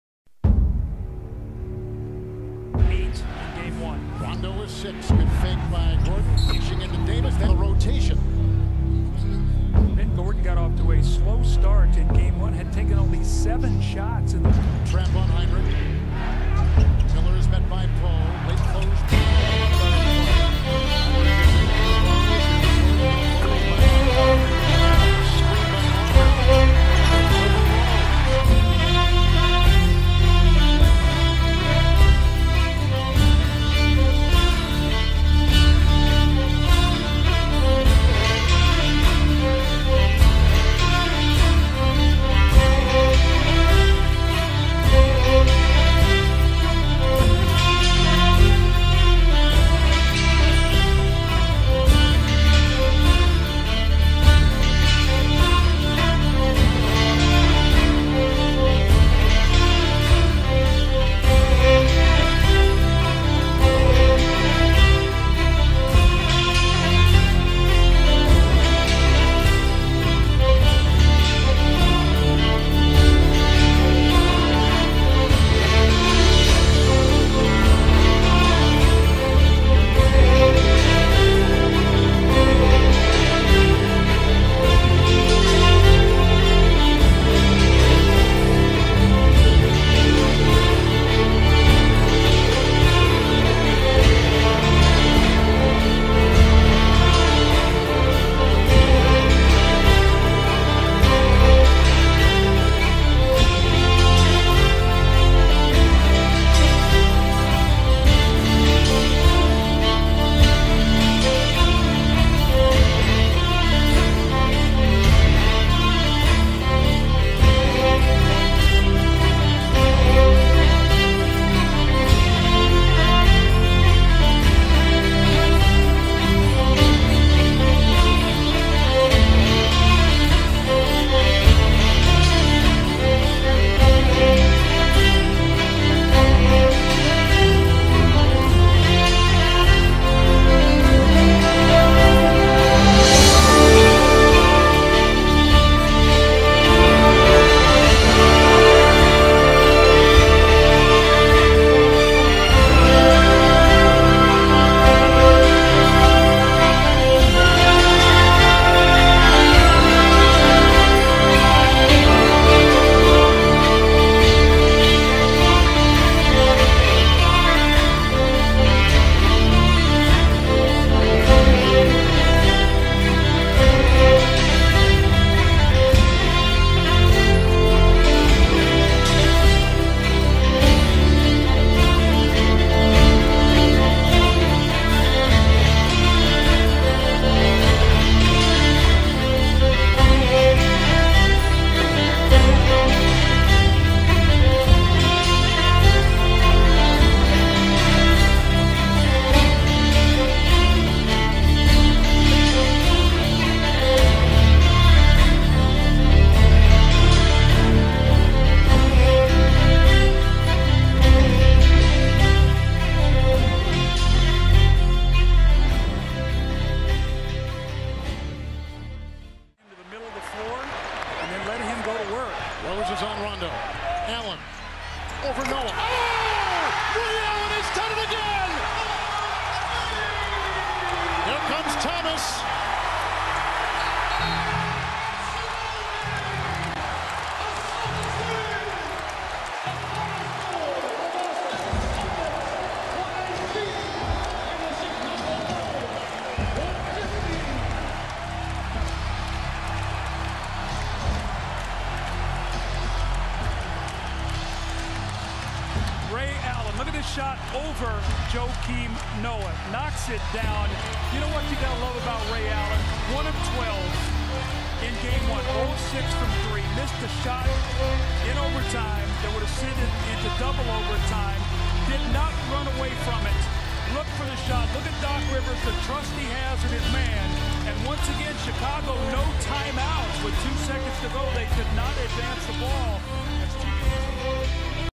[HD] Ray Allen vs Ben Gordon EPIC Duel [2009 Playoffs game 2]
Ray Allen & Ben Gordon ( Celtics vs Bulls ) duel in Game 2 of 2009 NBA Playoffs